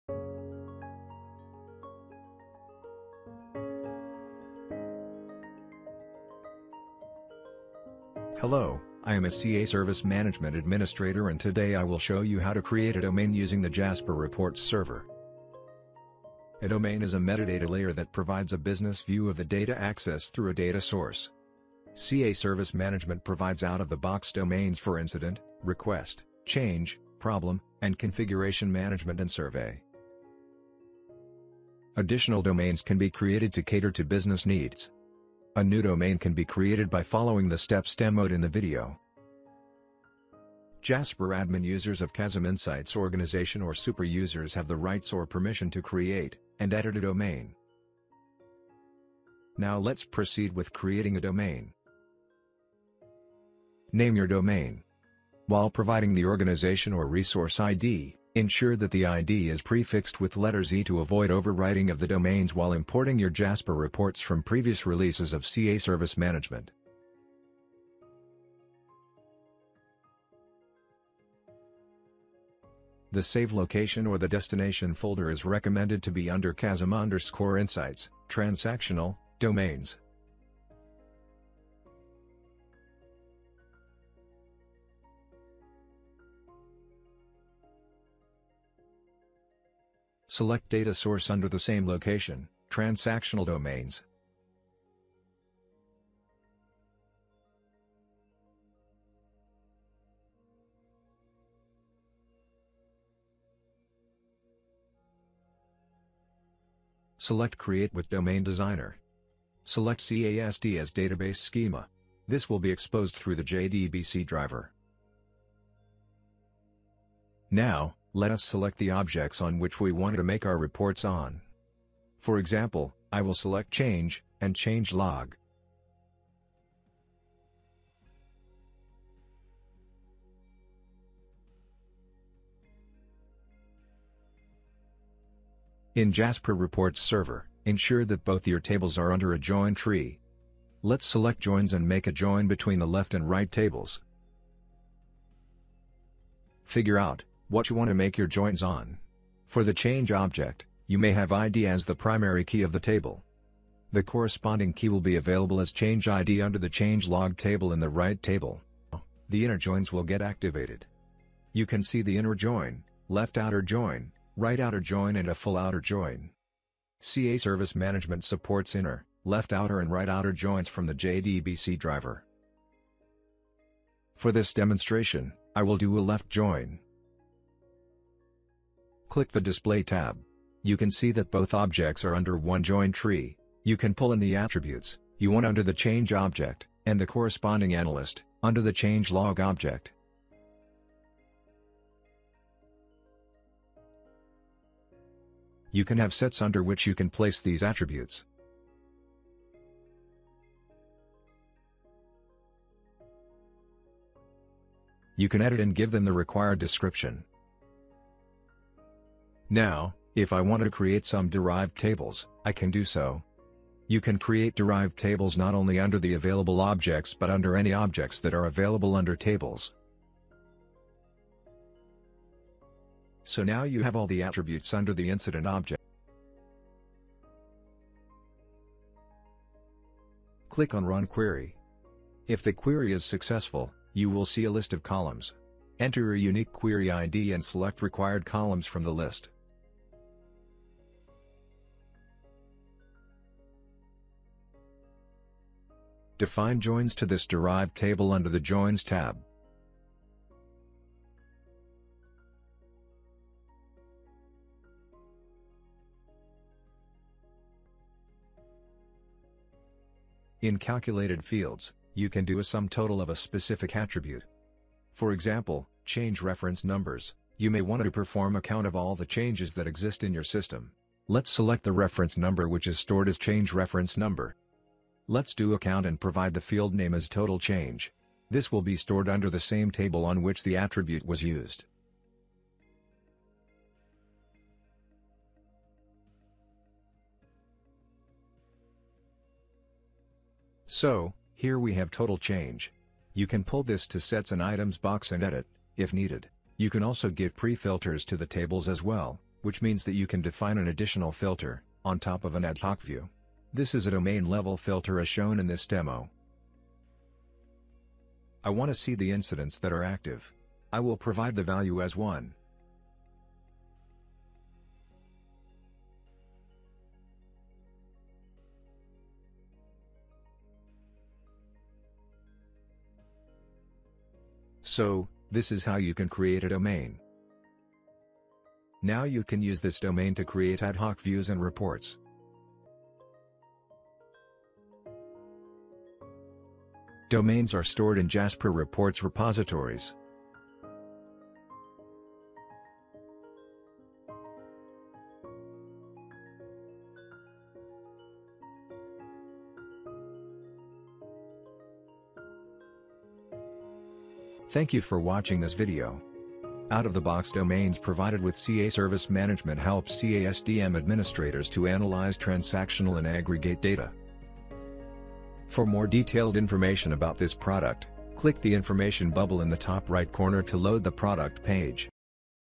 CA Service Management: Create a Domain in JasperReports Server
This video demonstrates how to create a domain in CABI JasperReports. Domains for Incident, Request, Change, Problem, Survey, and Configuration Management are available to help analyze CA SDM’s transactional data.